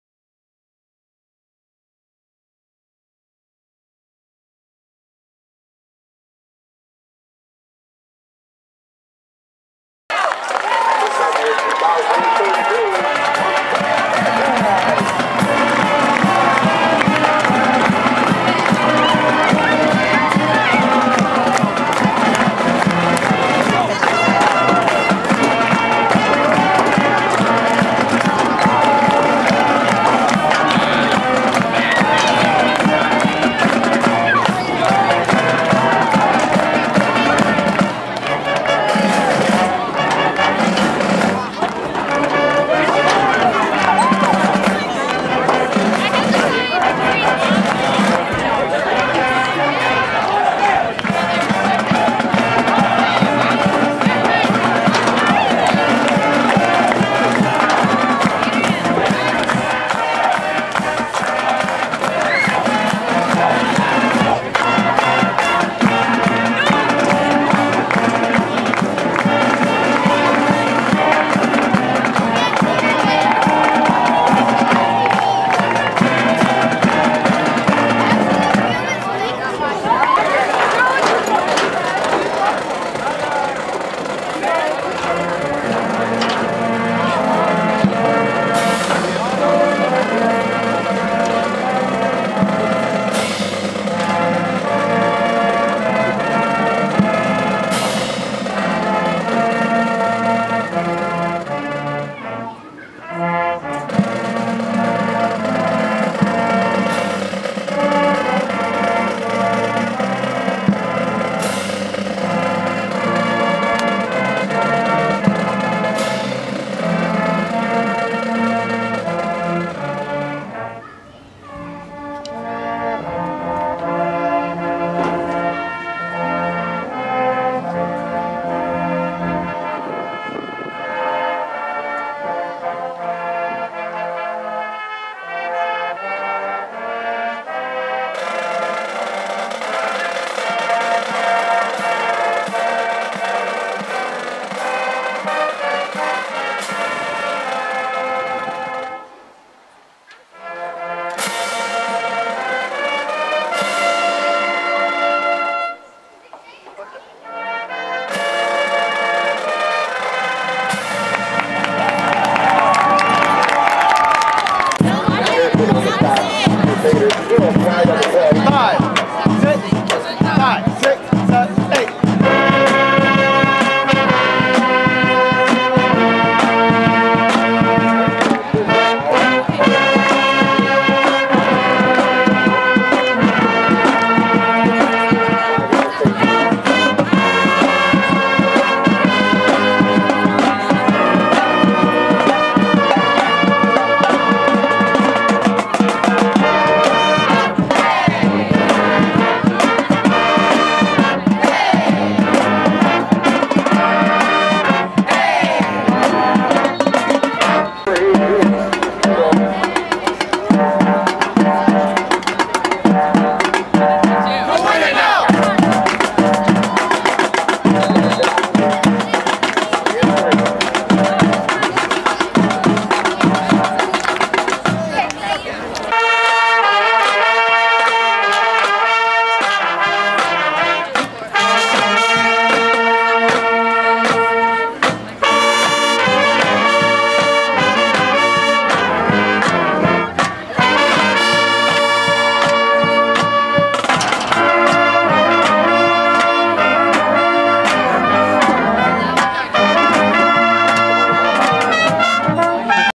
Fruitport High School - Pregame and Stands vs. MCC - 10/11/13
Fruitport Marching Band - MCC pregame and Stand videos